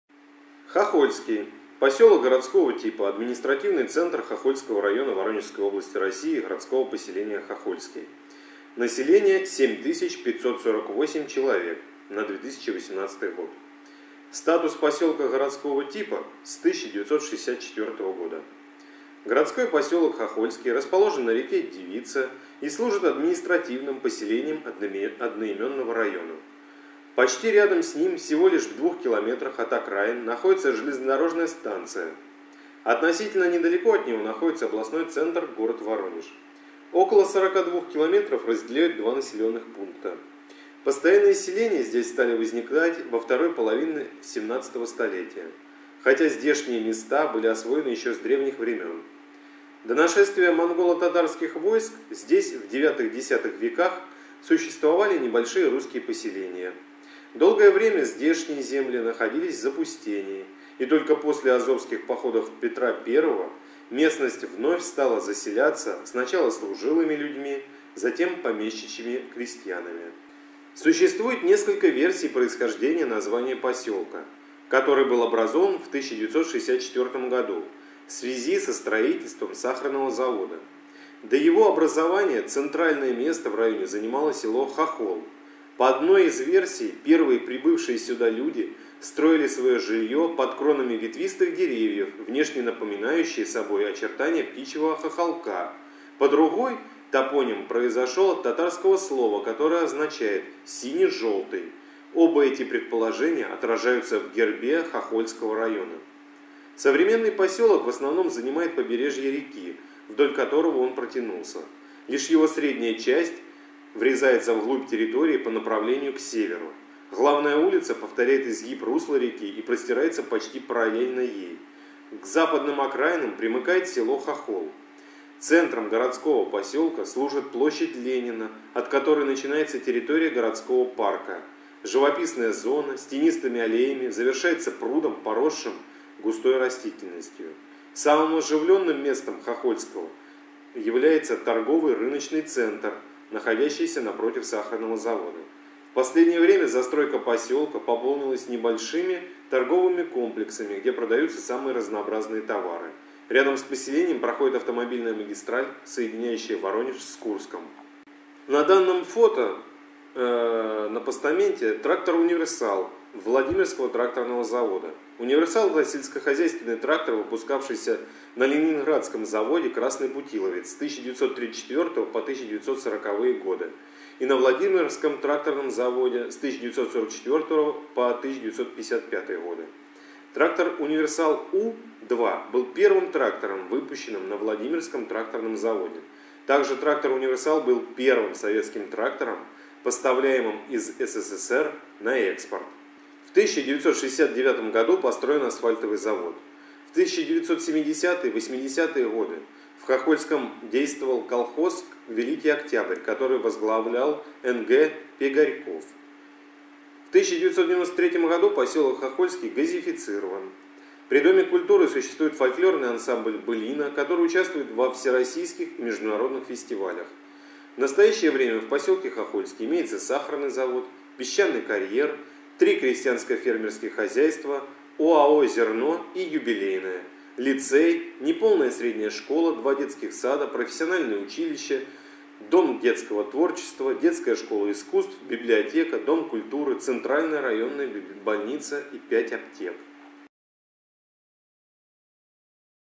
пгт. Хохольский, 12.04.2020, Хохольский район рассказ о посёлке городского типа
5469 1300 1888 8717

Поездка по посёлку, интересные достопримечательности, сравнительного молодого посёлка